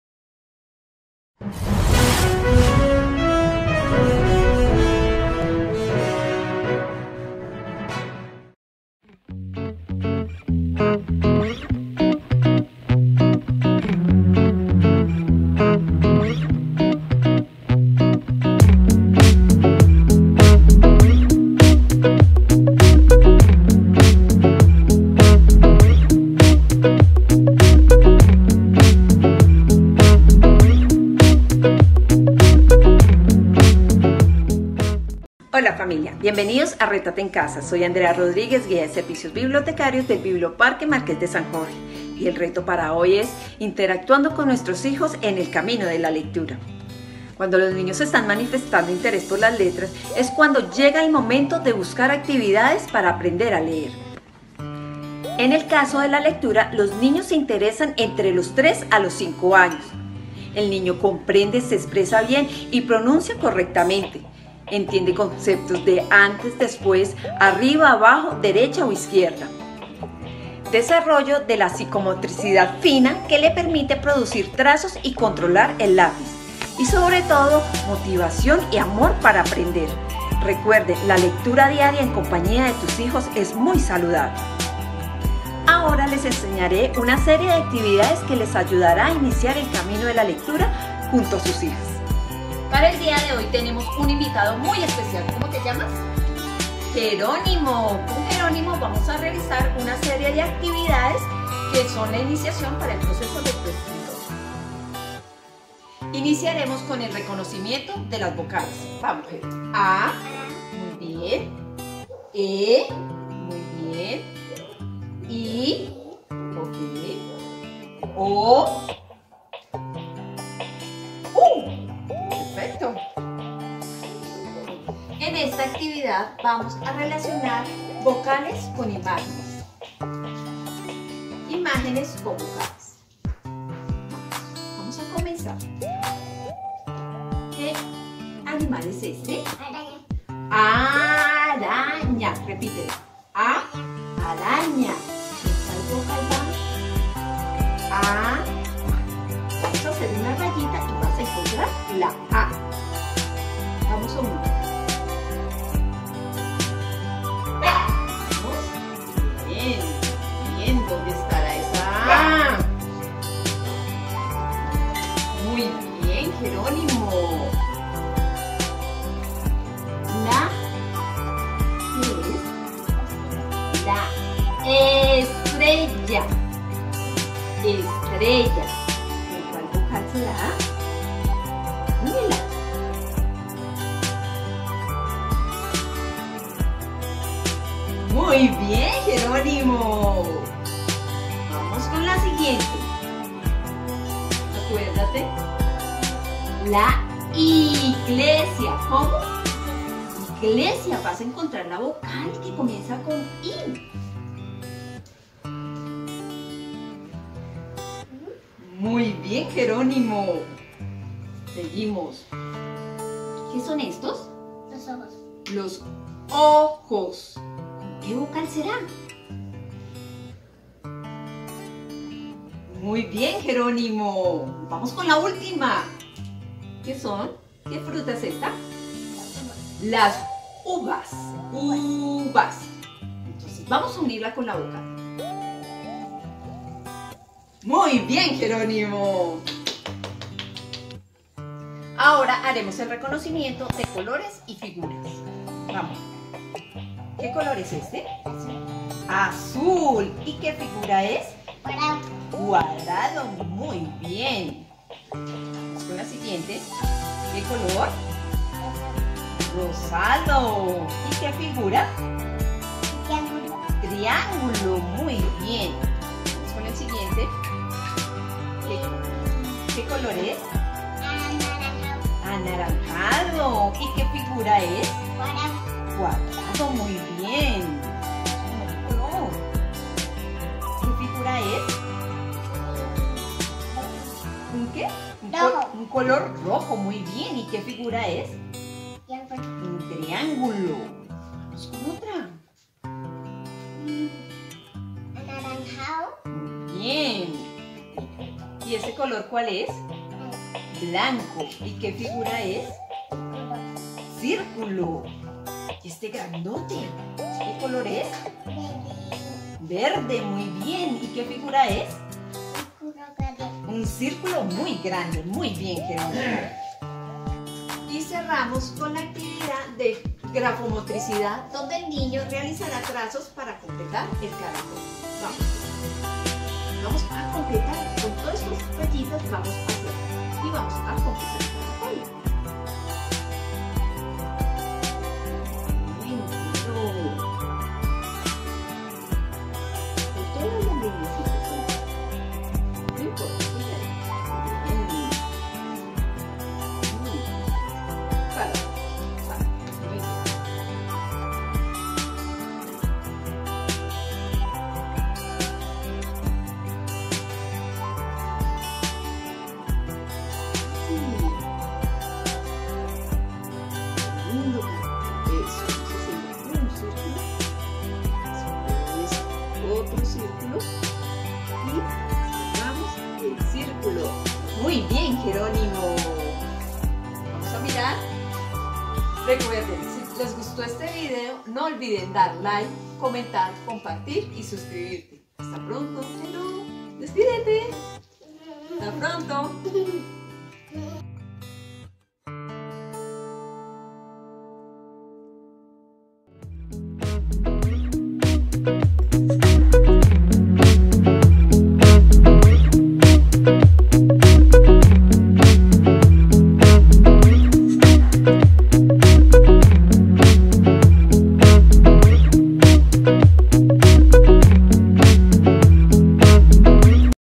BIBLIOPARQUE MARQUÉS DE SAN JORGE - RETO LECTURA [APRENDIZAJE EN CASA]
Descubre la magia de la lectura y viaja con nosotros a través de retos creativos que potencializaran tus habilidades lectoras.

No olvides:

Darle like 👍🏻
Comentar🗣
Compartir 📢